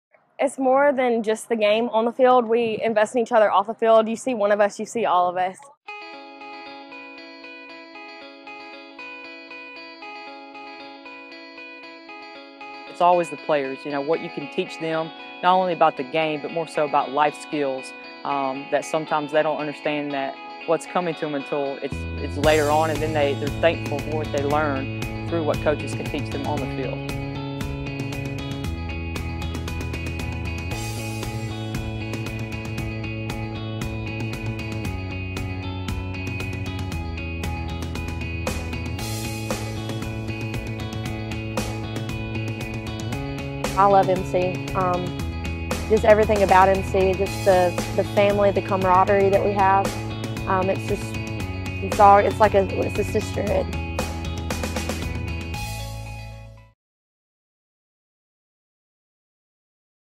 Season Ticket: Lady Choctaws Softball - Episode 5
Go inside Mississippi College Lady Choctaws softball all season with this special series.  Episode 5 describes what it takes to make the Lady Choctaws the 2016 NCCAA National Champions!